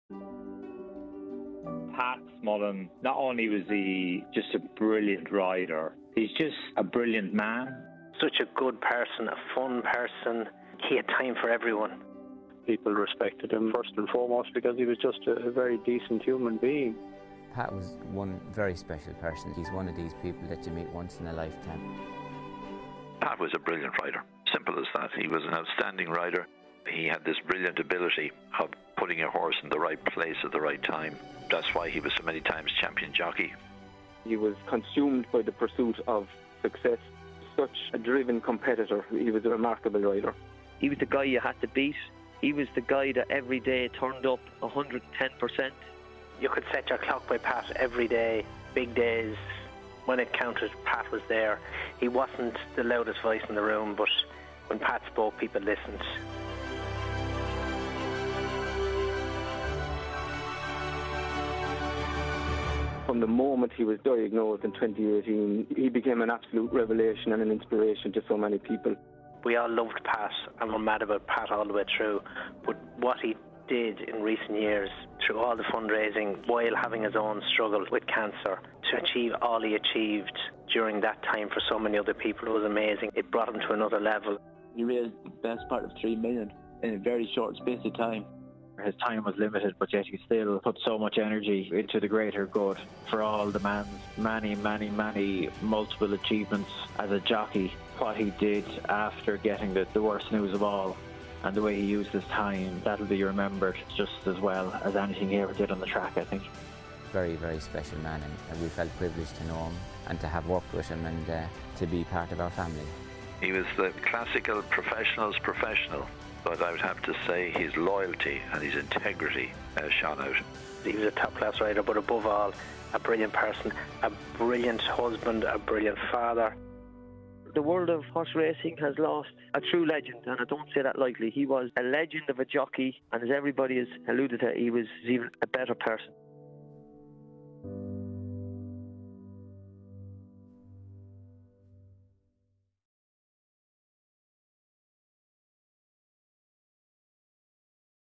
Pat Smullen | Tributes to a hero
An incredible rider. An incredible man. An inspiration
Rest in peace, Pat Smullen.

Racing's biggest names and some of Pat Smullen's closest friends including Johnny Murtagh, Barry Geraghty, Mick Fitzgerald, Sir AP McCoy and Mick Fitzgerald pay their tributes.